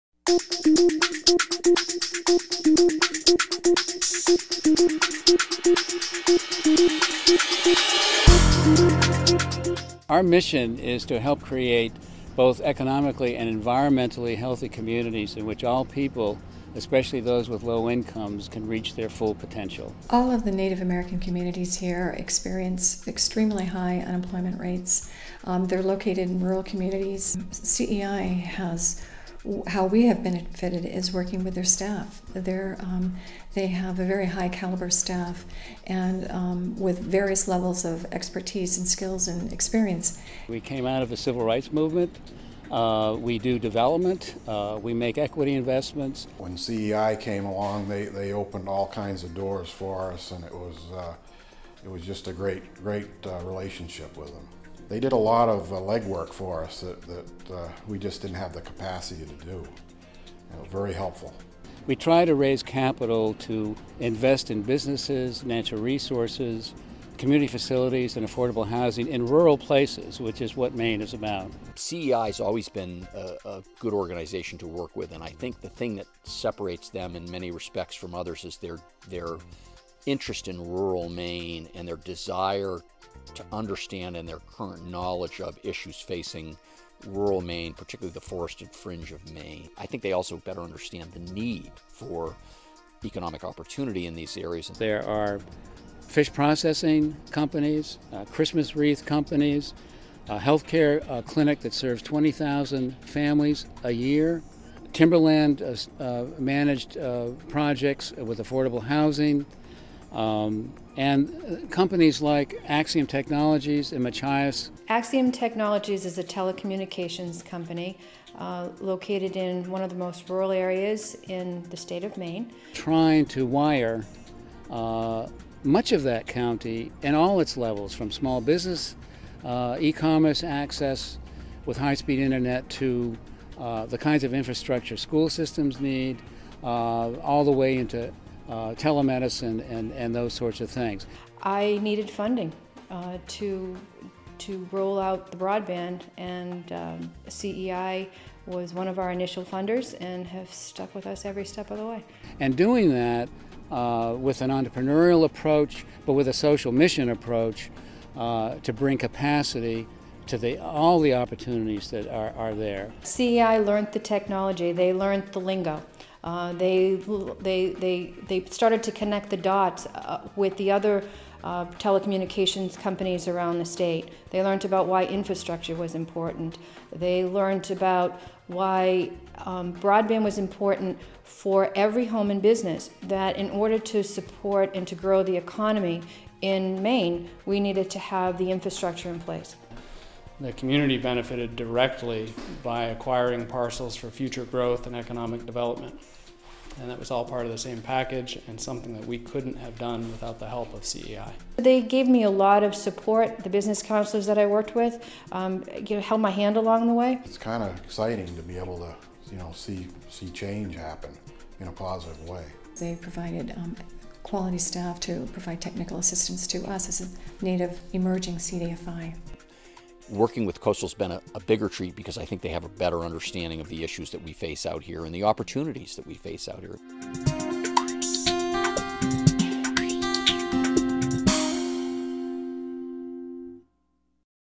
CEI PSA YouTube
Public Service Announcement for Coastal Enterprises Incorporated in Maine